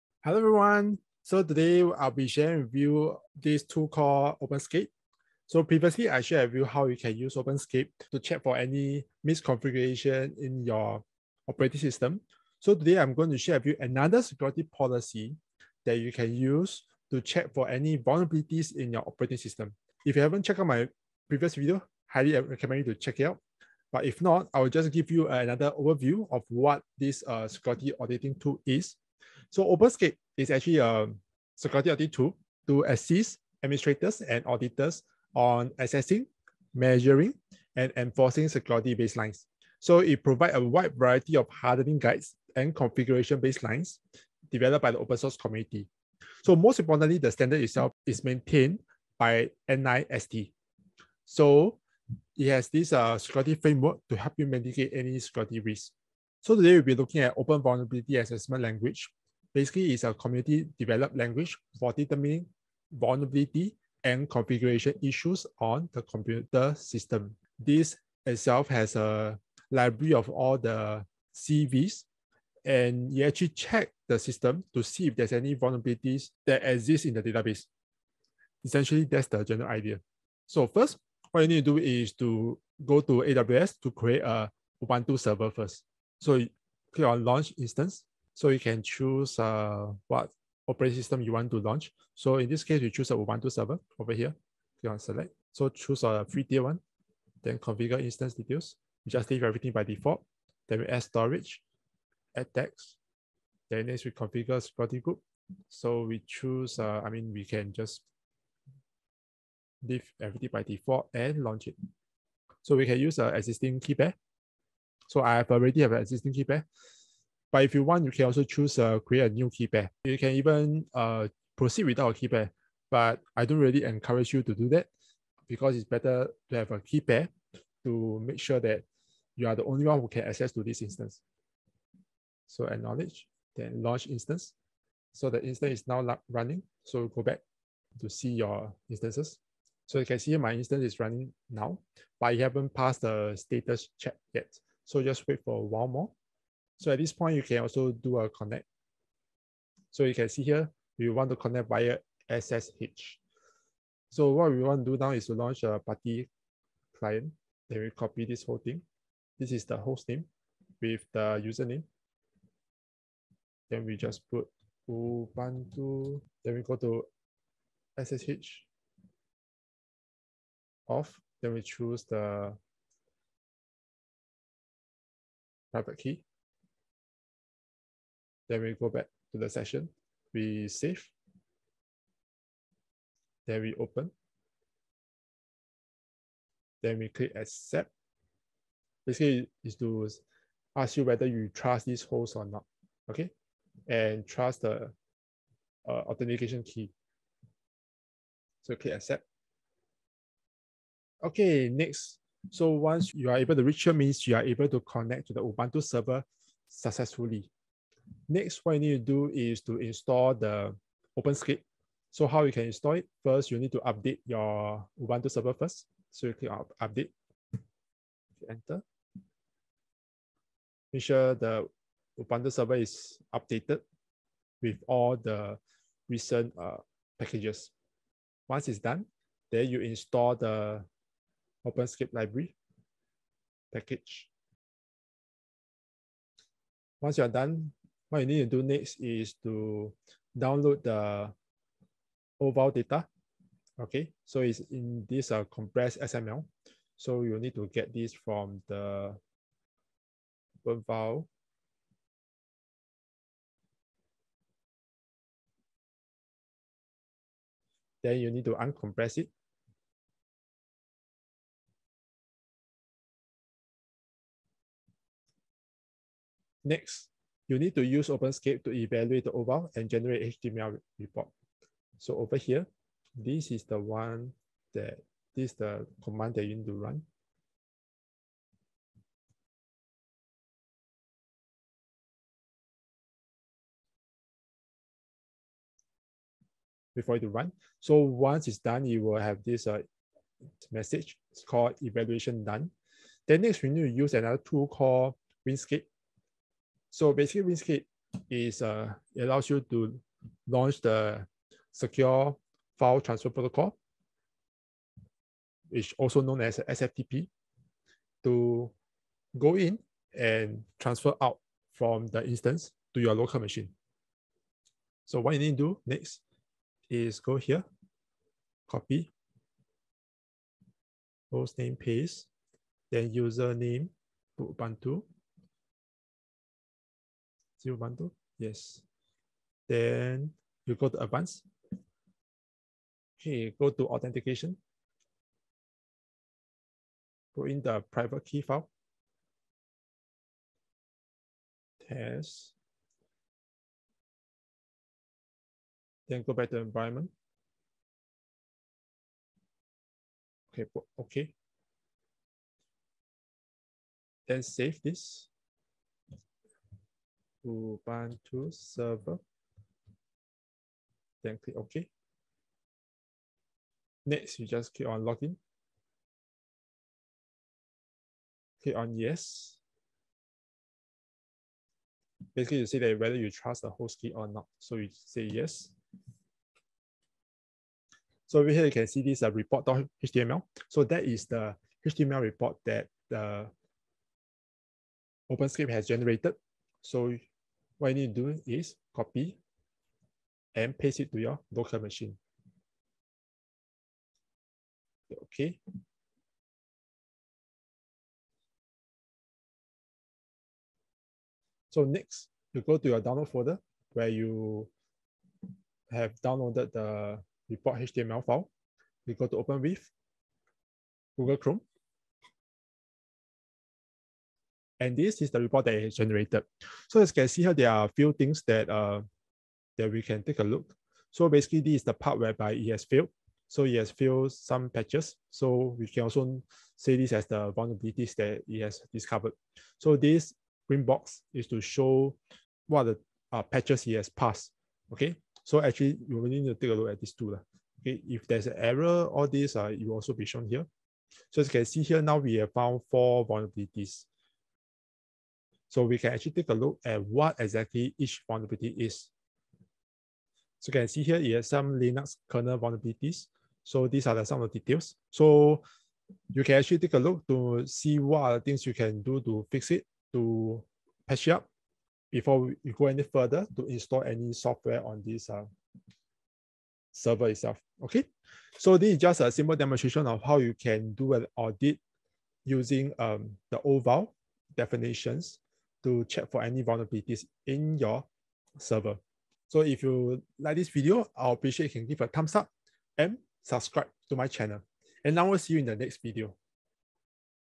How to generate OVAL report using OpenSCAP in Ubuntu Server
In this video, I will be sharing with you how you can generate the OVAL report using OpenSCAP on Ubuntu server.

GEARS
Blue Yeti USB Microphone
ASUS Wireless Gaming Headset